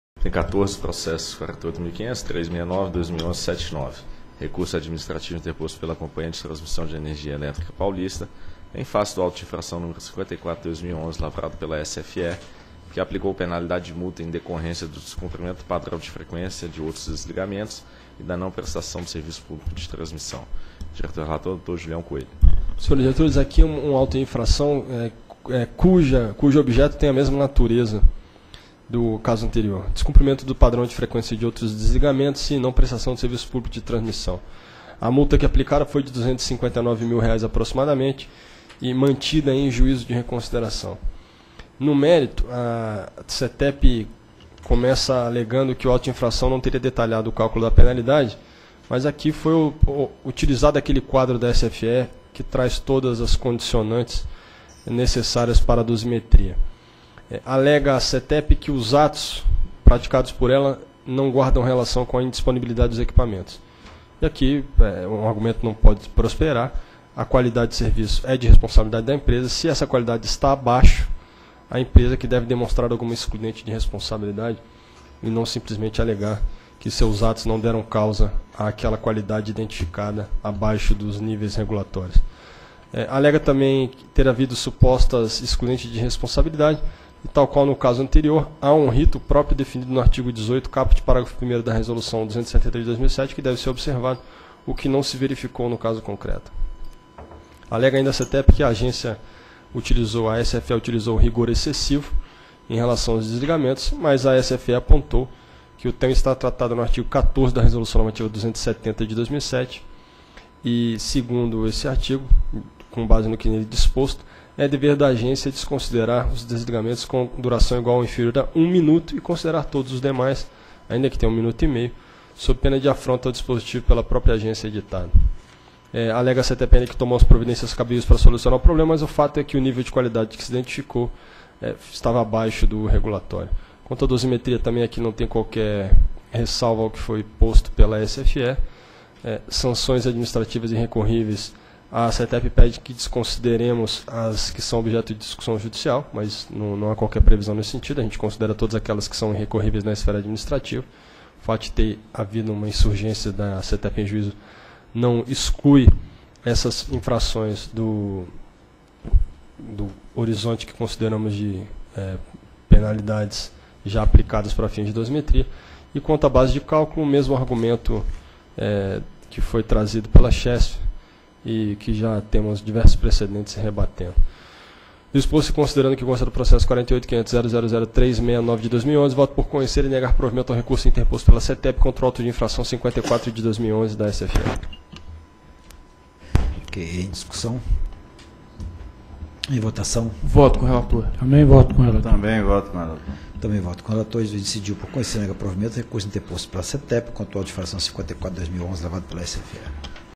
Item 14 - 2ª Reunião Pública Ordinária ANEEL 24/01/2012
Item 14. Processo nº: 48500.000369/2011-79. Assunto: Recurso Administrativo interposto pela Companhia de Transmissão de Energia Elétrica Paulista -- CTEEP em face do Auto de Infração nº 54/2011, lavrado pela Superintendência de Fiscalização dos Serviços de Eletricidade -- SFE, que aplicou penalidade de multa em decorrência do descumprimento do Padrão de Frequência de Outros Desligamentos e da não prestação do serviço público de transmissão. Área Responsável: Diretoria Geral -- DIR.